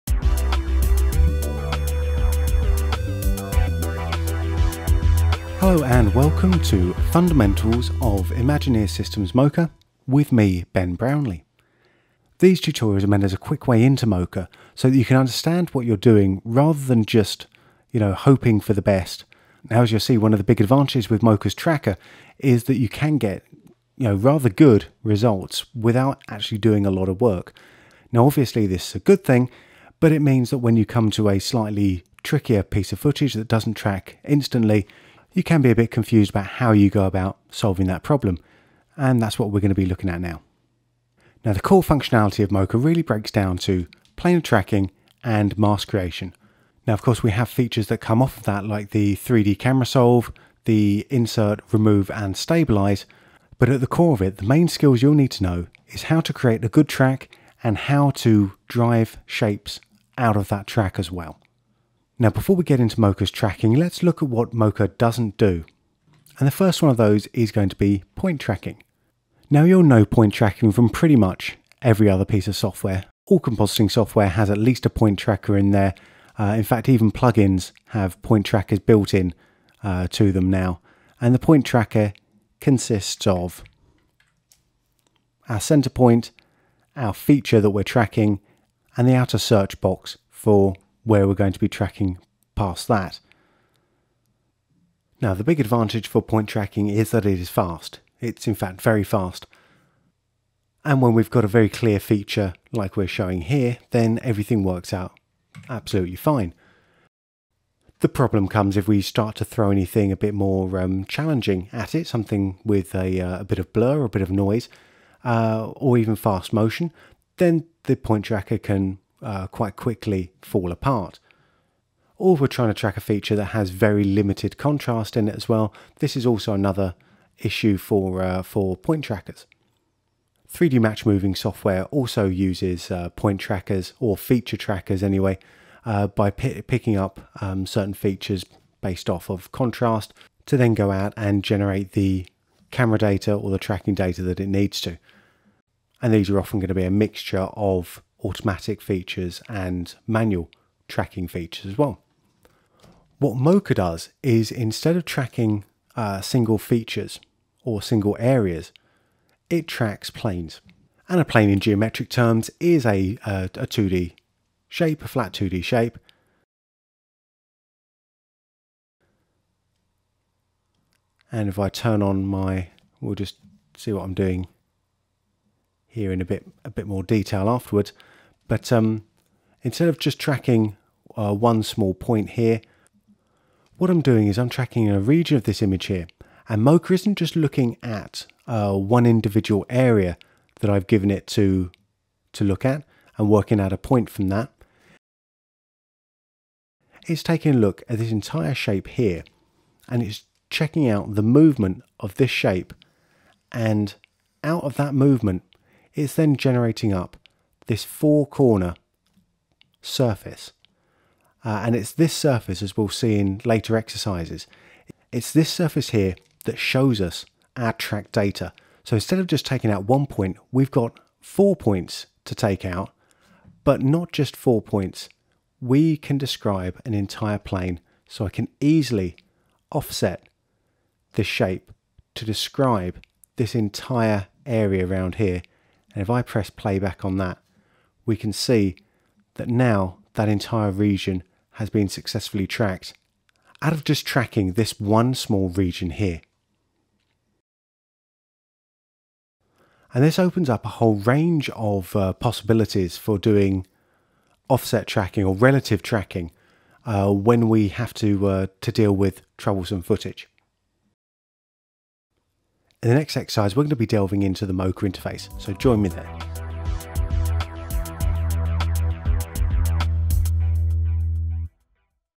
Fundamentals of Mocha - 01 - Fundamentals of Tracking
What you'll learn:
• A look at traditional tracking techniques
• What mocha doesn't do
• The first Step into Planar Tracking

A short exercise mainly looking at point and feature trackers, to show what mocha does not use.  We then take a brief look at mocha's Planar tracker and learn about the separation of Shape and Tracking data.